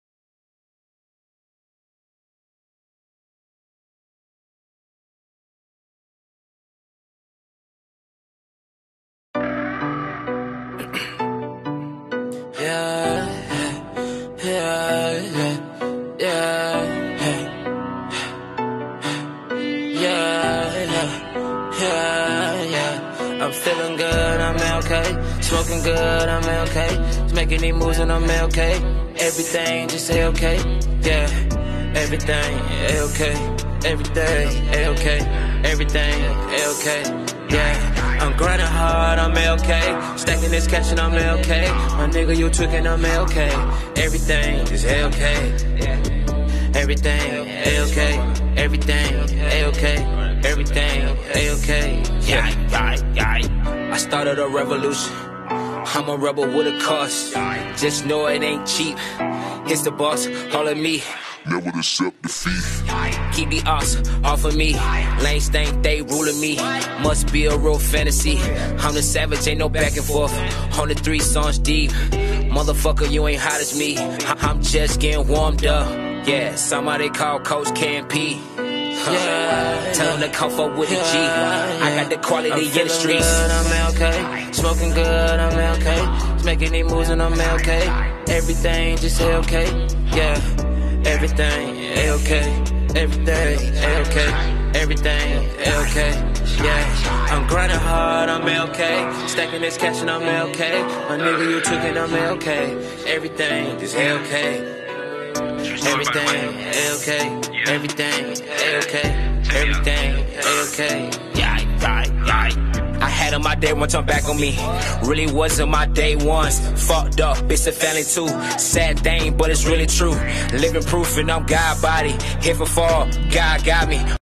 RECO SAVAGE LIVE PERFORMANCE RECAP AT THE THIRSTY TURTLE
Recap of BOS LIFE ENTERTAINMENT artist Reco Savage live performance at thirsty turtle in Macon,ga